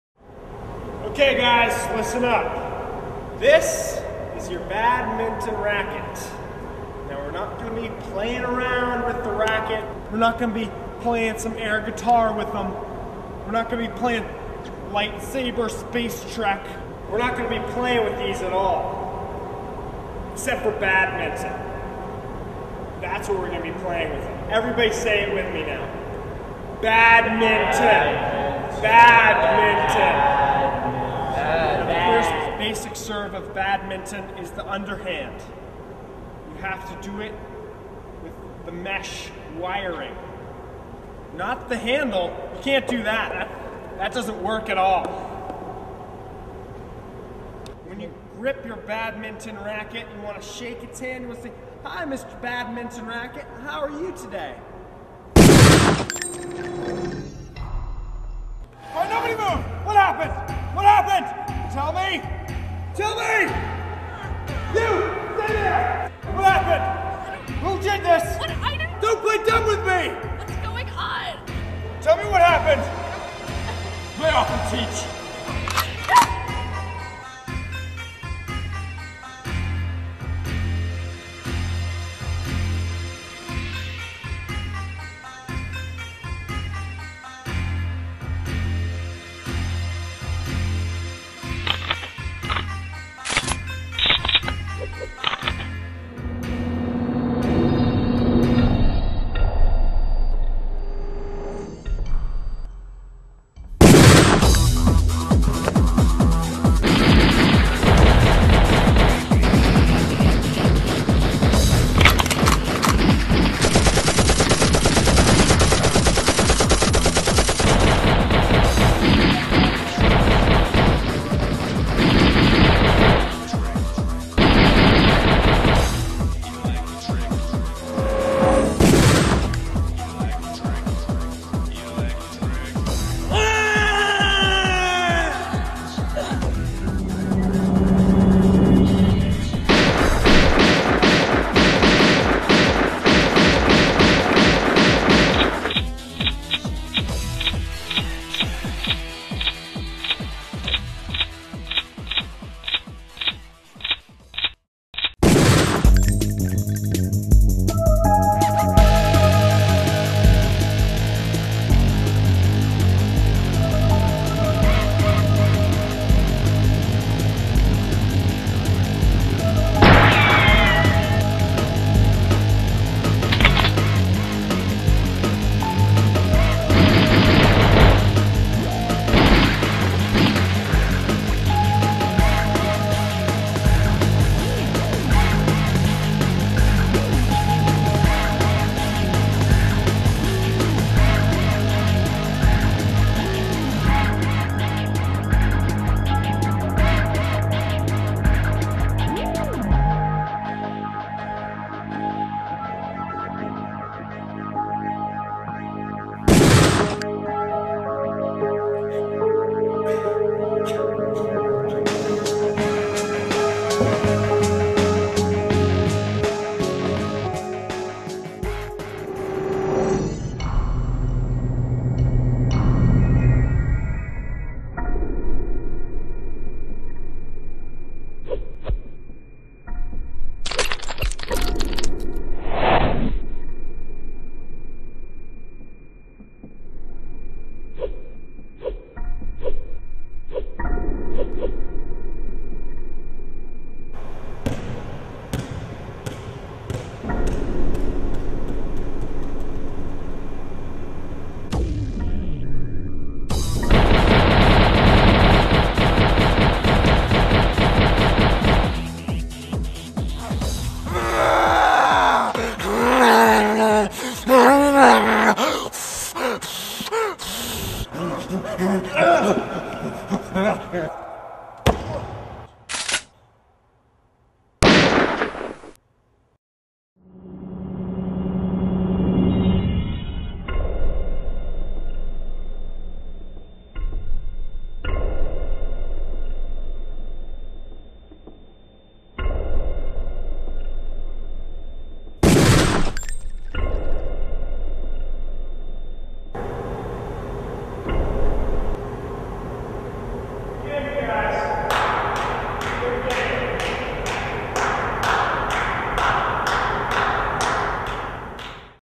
Gym Class - Olde English Sketch Comedy
Gym Class was created in the fall of 2003.  All of the blood effects in this video were created by Amy Searles except for the minor cuts on Jon Dame's face (the last person to get shot).  Those are from him attempting a backflip while intoxicated a few days earlier.